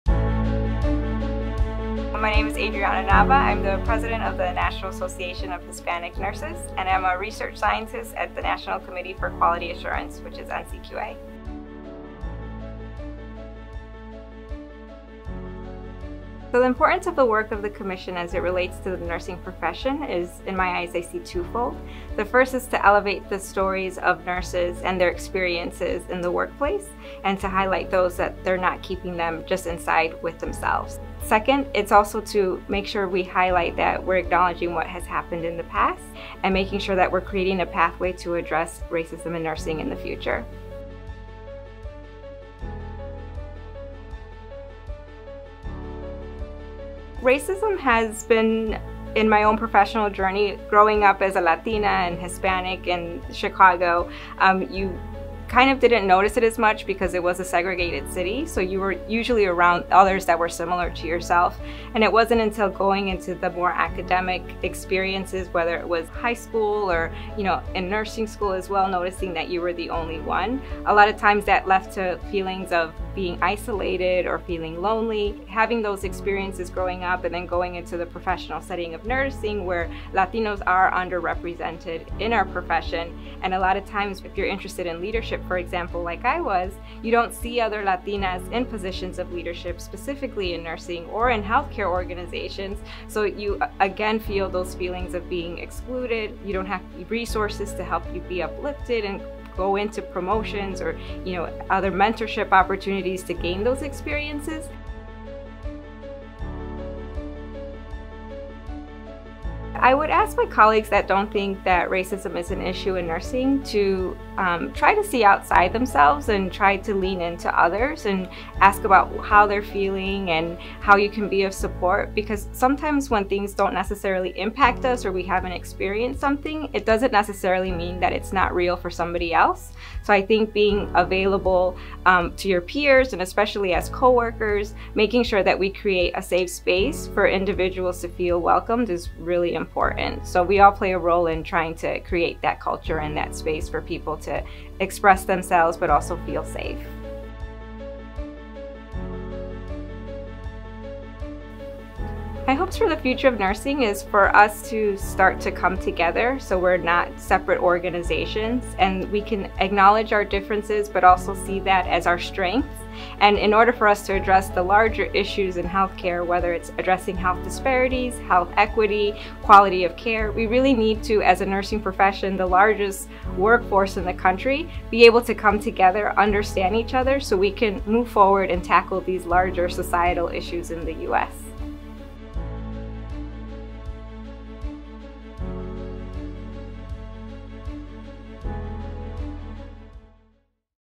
Nursing & Racism: A Conversation w/ Adrianna Nava, President, National Association Hispanic Nurses
Discover how racism is playing a role in the nursing profession, in this essential interview with Adrianna Nava, President of the National Association of Hispanic Nurses and co-lead of the National Commission to Address Racism in Nursing. Hear how nurses and nurse allies can take action to support fairness, equity, and antiracism.

Dive into the foundational report to learn more about this critical issue facing our profession today

For more information on how you can be an ally in nursing